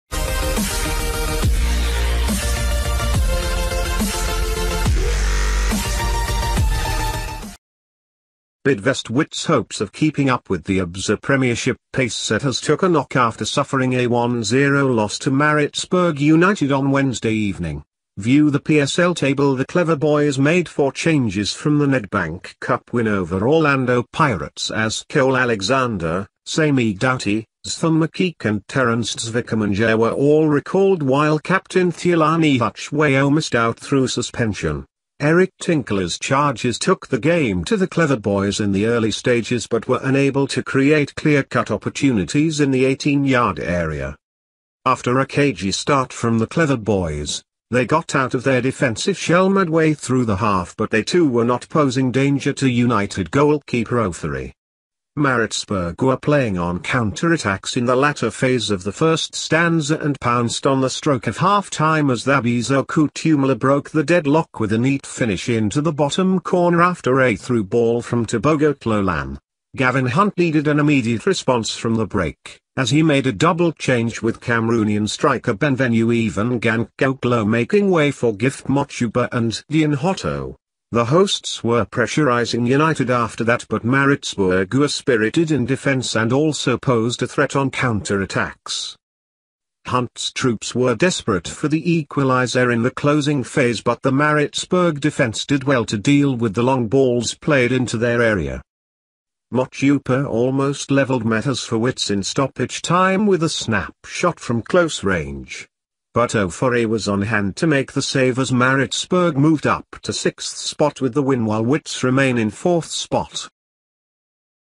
Absa Premiership: Maritzburg Dent Wits' Title Aspirationss l South African Soccer News
Absa Premiership:  Maritzburg Dent Wits' Title Aspirationss

Bidvest Wits' hopes of keeping up with the Absa Premiership pace-setters took a knock after suffering a 1-0 loss to Maritzburg United on Wednesday evening.

South African soccer news - Channels update soccer south africa !